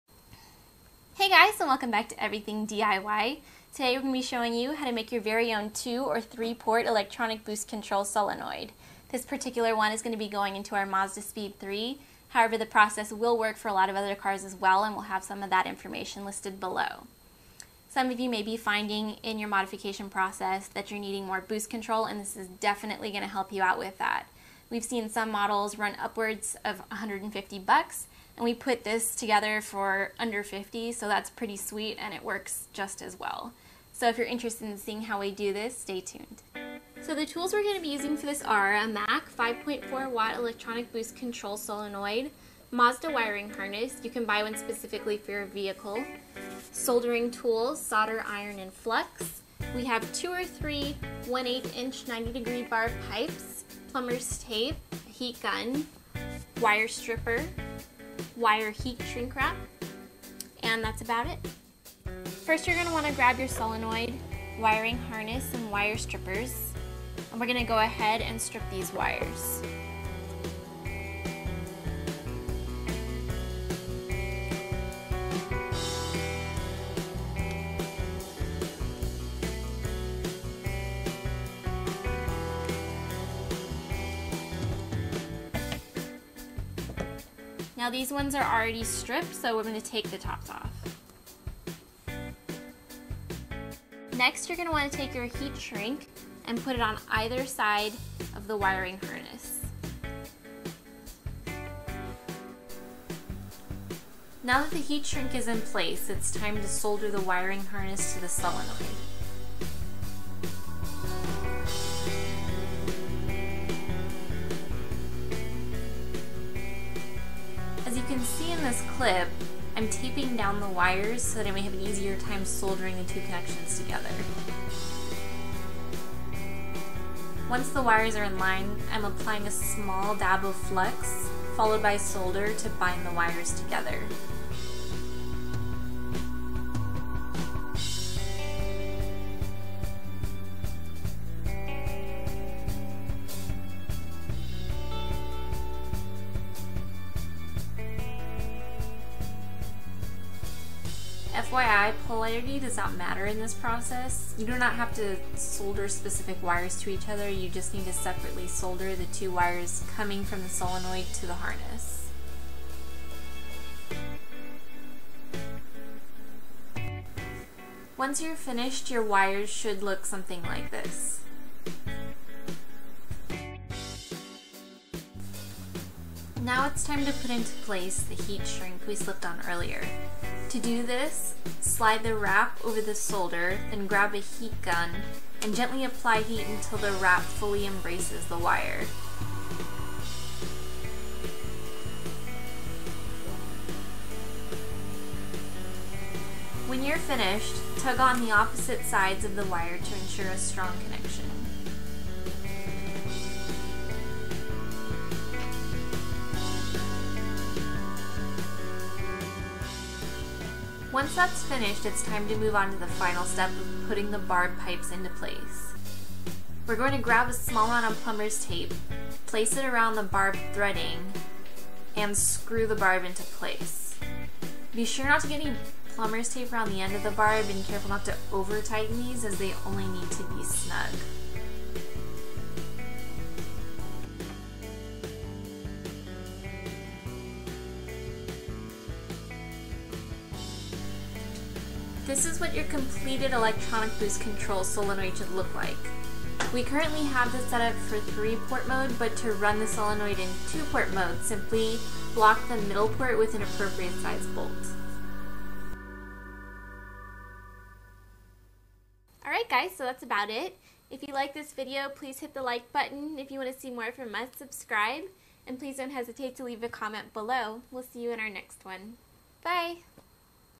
DIY ELECTRONIC BOOST CONTROL SOLENOID!
Today We Make Our Very Own Electronic Boost Control Solenoid!

This task is very simple- all you really need is: the correct wiring harness, the right wattage solenoid, and a few barb pipes. After that, it's simple splicing and soldering. This is pretty much the same product you are buying from other manufacturers like Cobb or Grimmspeed, but at a third of the price. This valve will work just as well due to the fact that it uses the same MAC EBCS that most manufacturers use.

Tools Used:
- Soldering Tools (Solder, Iron and Flux)
- Wire Stripper
- Plumbers Tape
- Heat Gun

Products Used
- MAC 5.4 Watt EBCS
- Mazda Wiring Harness
- 1/8 inch 90 Degree Barb Pipes
- Heat Shrink Wire Wrap

The techniques shown in this video apply to various turbo vehicles that include, but are not limited to the following:
Mazdaspeed3
Mazdaspeed6
WRX
WRX STI
EVO
GENSIS

Specific Part Numbers for Products Used in this Video:
-MAC 5.4 Watt EBCS (Part # 35A-AAA-DDBA-1BA)
-90 Degree Metal Barb (Part # ELB60-1/8)
-Mazda Wiring Harness (Part # CONN-85730)

The DIY rep in this video is Ally!

***SAFETY FIRST EVERY TIME***